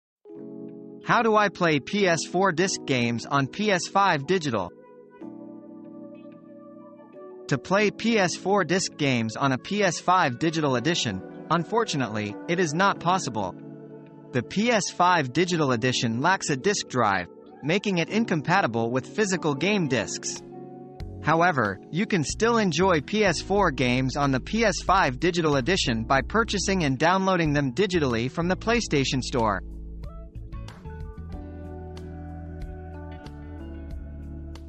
How do I play PS4 disc games on PS5 digital?
"Unlock the Full Potential of Your PS5 Digital Edition | Discover How to Play PS4 Games Without Discs! 🎮🔥"

Looking for a way to enjoy your favorite PS4 games on your PS5 digital edition? We've got the answer! 🙌🏼🎮

In this exciting video, we'll guide you through the process of playing PS4 disc games on your PS5 digital edition. While the lack of a disc drive may seem like a setback, fear not! We'll show you how to still dive into the immersive world of PS4 gaming on your sleek PS5 digital edition. 💥

Say goodbye to physical game discs and embrace the digital revolution! Discover the convenience and endless possibilities of purchasing and downloading PS4 games directly from the PlayStation Store. 🌟🛒

Join us as we unravel the secrets to unlocking the full potential of your PS5 digital edition. Get ready to immerse yourself in stunning graphics, epic adventures, and unforgettable gameplay experiences without ever needing a physical disc! 🌌🚀

Don't miss out on the action! Click play now and let us guide you towards an incredible gaming experience on your PS5 digital edition. Get ready to level up and embark on thrilling virtual journeys like never before! 🎉✨